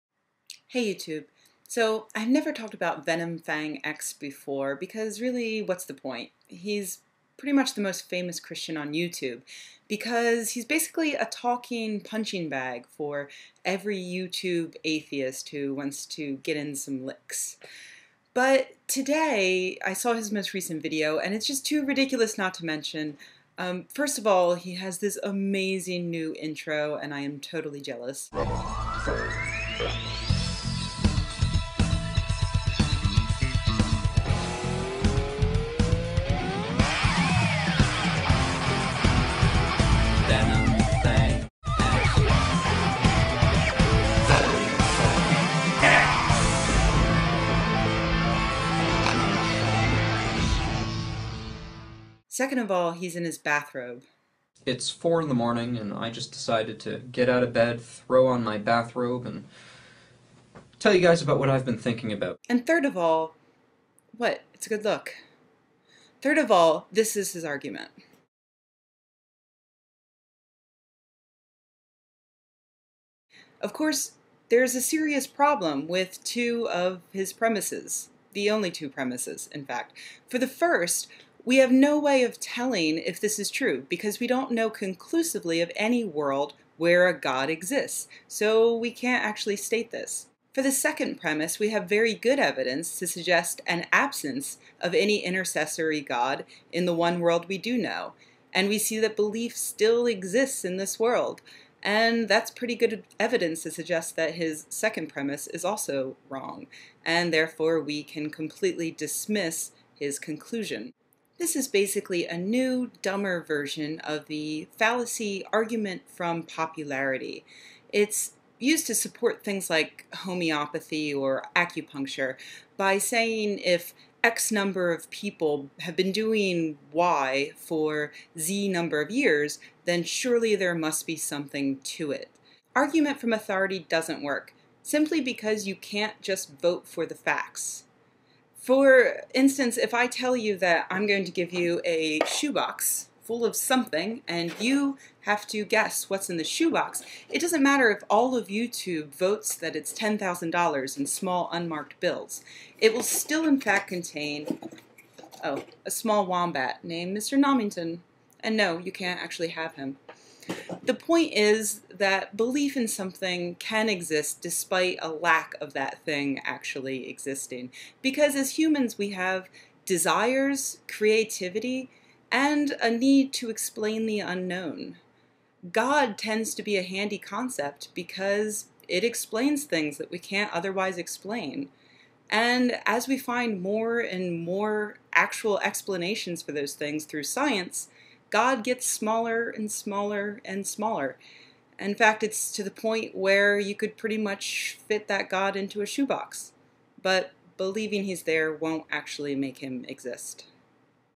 Venom Fang X and the Argument from Popularity
I've never really done a video about a YouTube creationist before. Exciting! Probably won't make a habit of it. But anyway, this gives us a good opportunity to talk about the Argument from Popularity or argumentum ad populum or appeal to the majority. Here's VFX's original video if you'd like to see the whole thing for yourself: